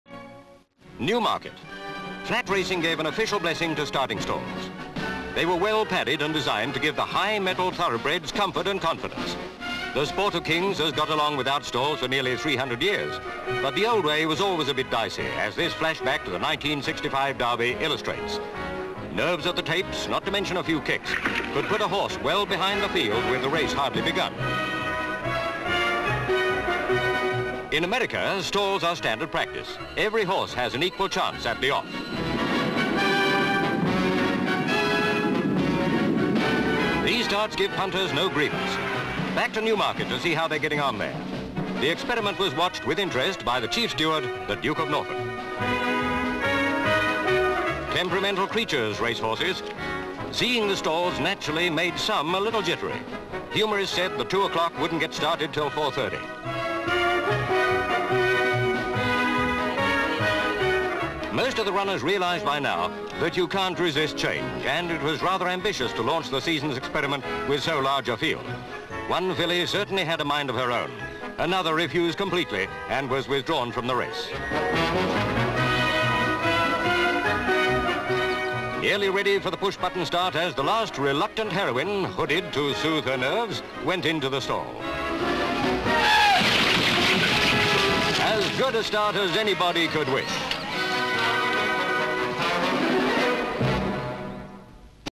New Starting Stalls (1967)
No title - New starting stalls introduced at Newmarket racecourse.  Newmarket, Suffolk.

Various shots of the new starting stalls at Newmarket.  C/U as man closes gate. M/S flashback to 1965 Derby with horses standing behind wire, M/S as they set off.  L/S as American race begins with horses in starting stalls.  Various shots as horses prepare to enter starting stalls at Newmarket. M/S Duke of Norfolk watching. Various shots as they enter the stalls, one of them goes into stall and then backs out again. L/S and M/S from the front of the stalls as the horses get in. M/S as one horse refuses to enter stall. L/S from the front of the stalls. L/S as starter mounts steps. M/S as the last horse is got into stall with hood over its head. M/S as jockey takes hood off and throws it to somebody behind. M/S as the gates open and the race starts.
 FILM ID:2027.16